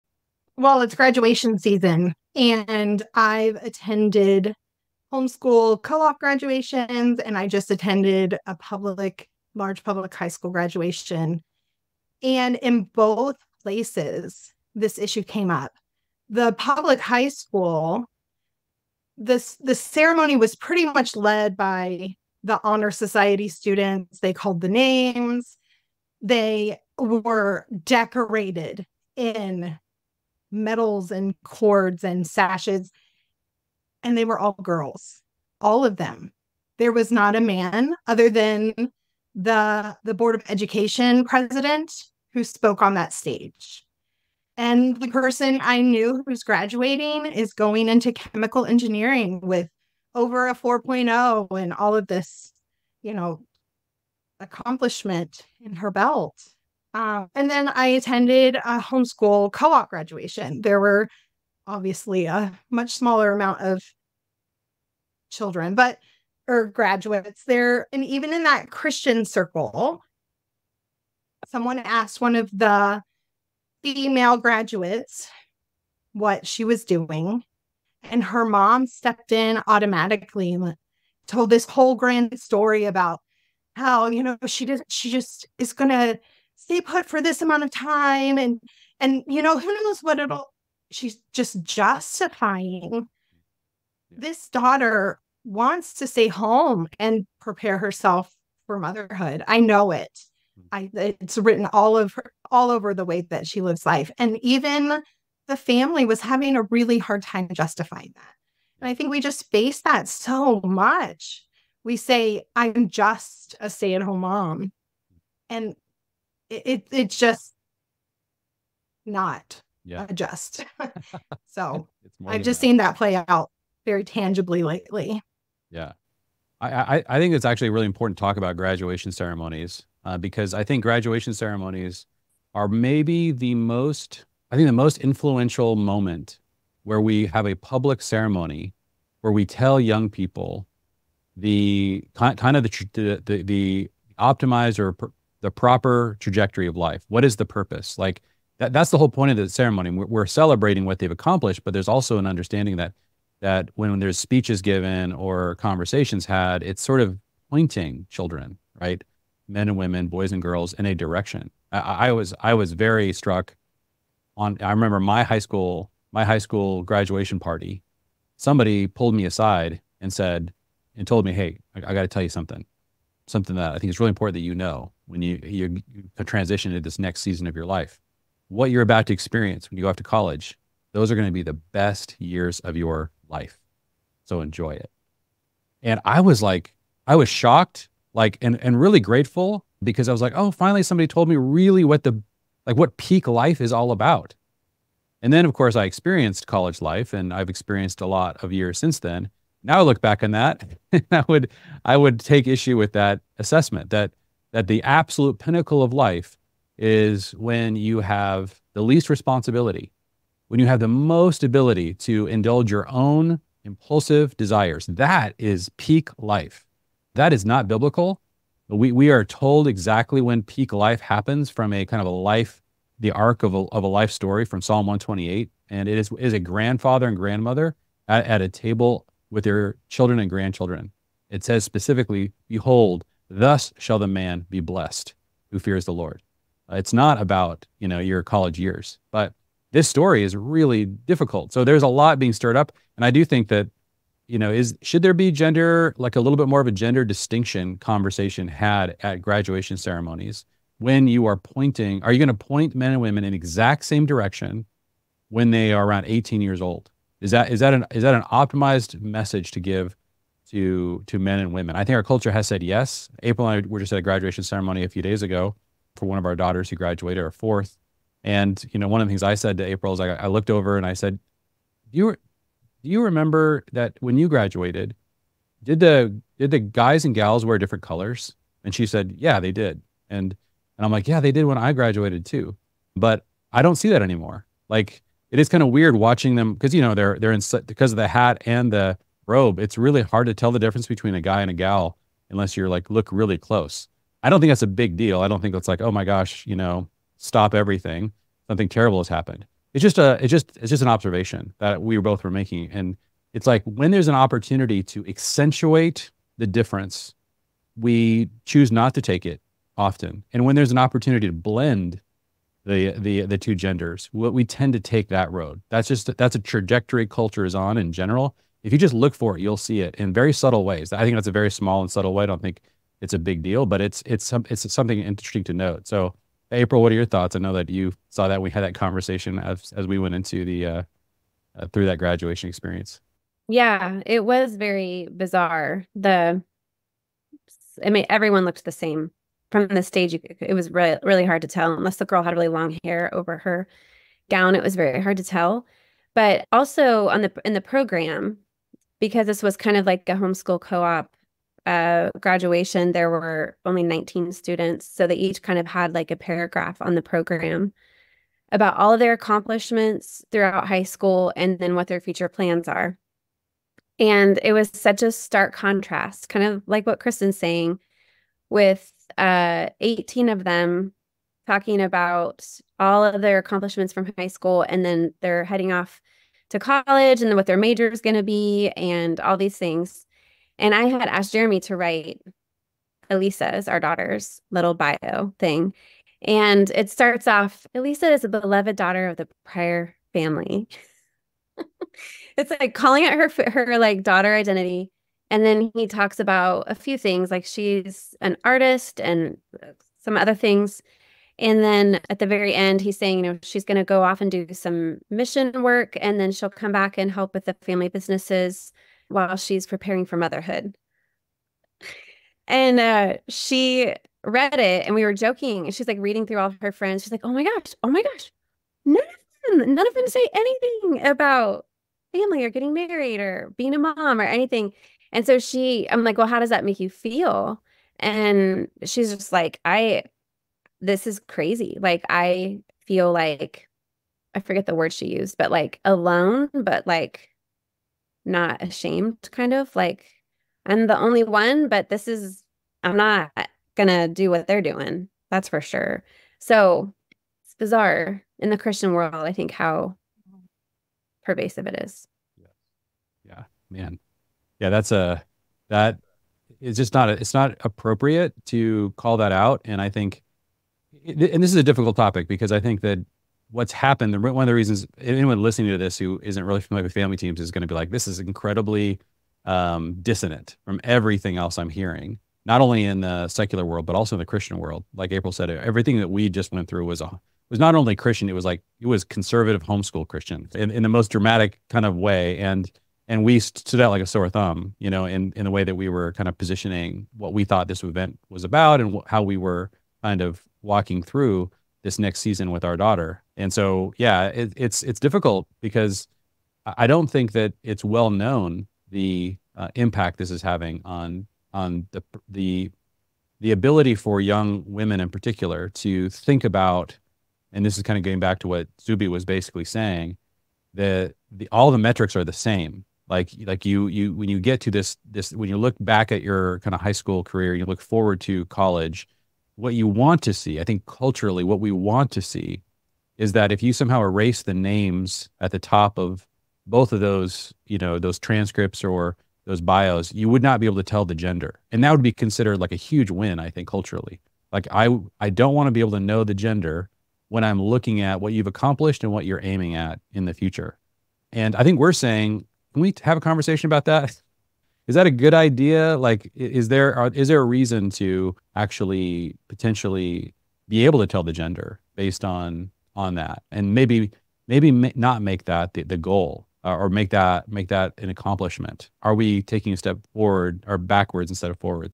What Graduation Ceremonies Teach Us About Gender and The War on Families
These days you can hardly tell whether the person graduating is a boy or a girl. And not only is that a sad commentary on the state of society, it stops us from performing a vital function in these ceremonies...telling kids what to do with their life next.

Because here's the thing...a boy and a girl SHOULD have different trajectories. But you're "not allowed" to say that anymore, and we're dooming our kids in the future.